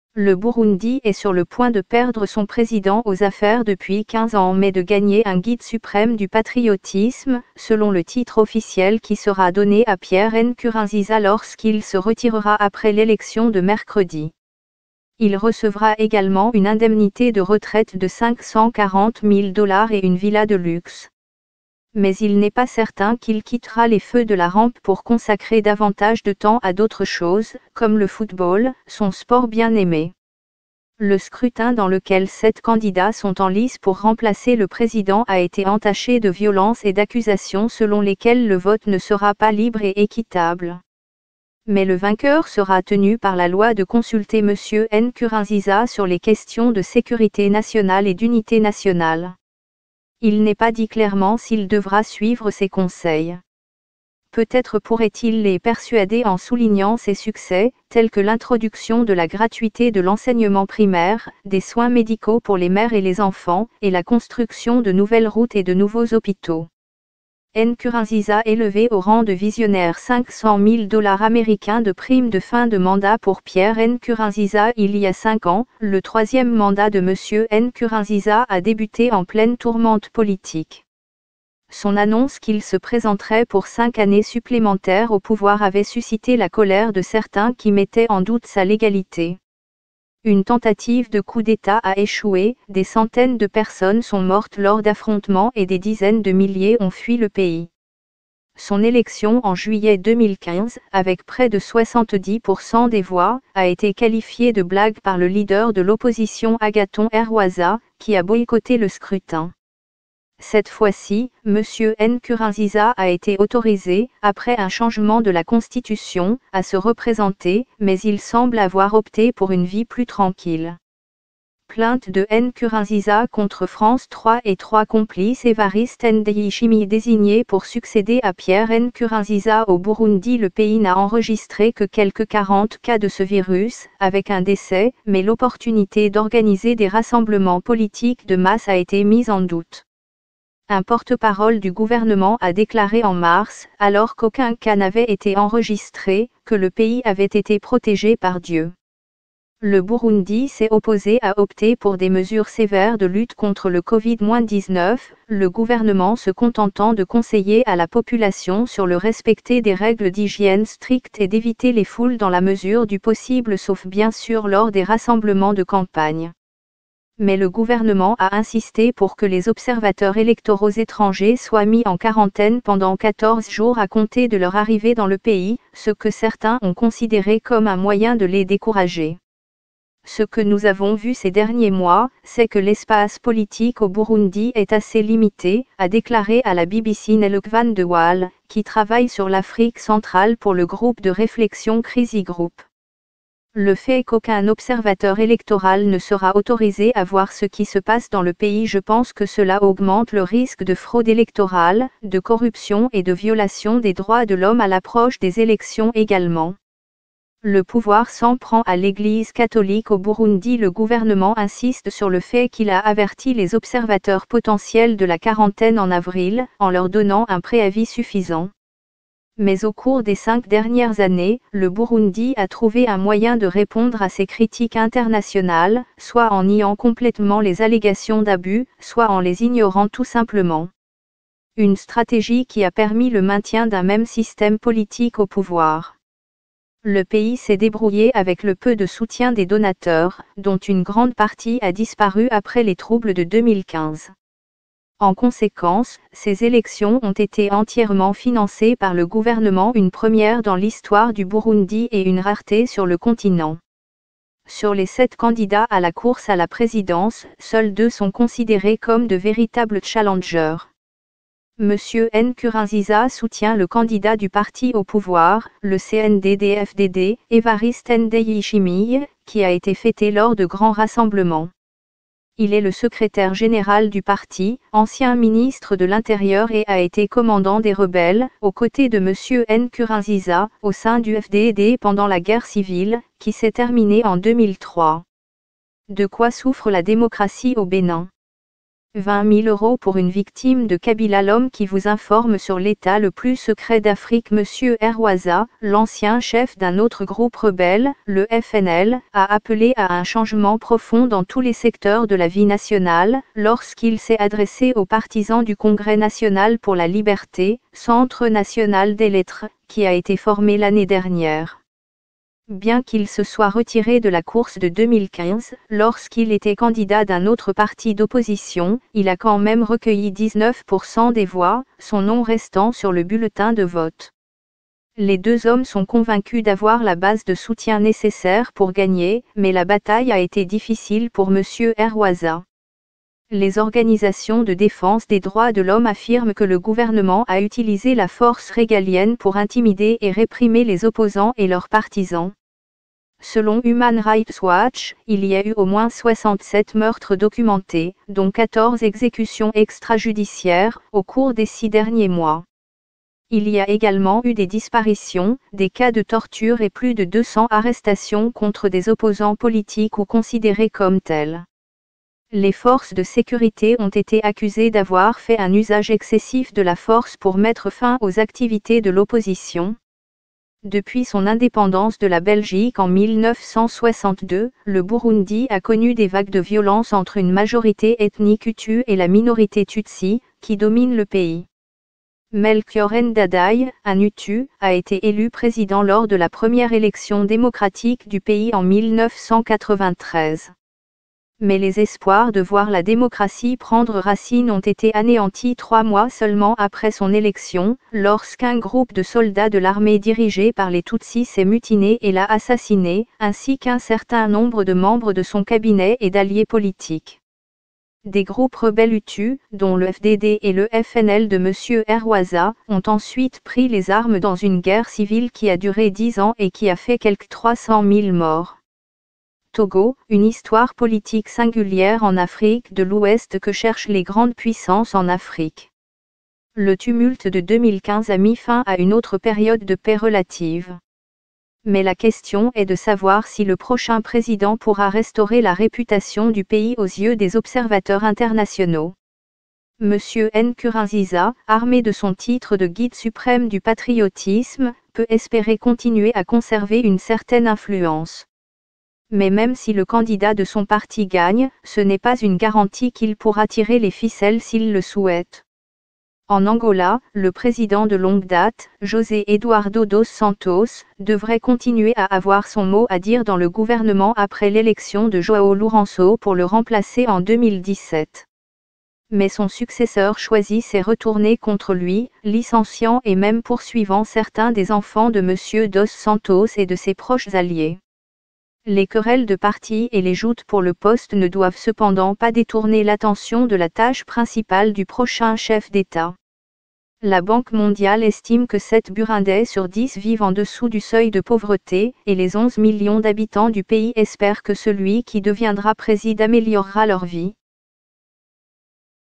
Élection au Burundi : Nkurunziza va devenir le "guide suprême"
Le Burundi est sur le point de perdre son président aux affaires depuis 15 ans mais de gagner un "guide suprême du patriotisme", selon le titre officiel qui sera donné à Pierre Nkurunziza lorsqu'il se retirera après l'élection de mercredi.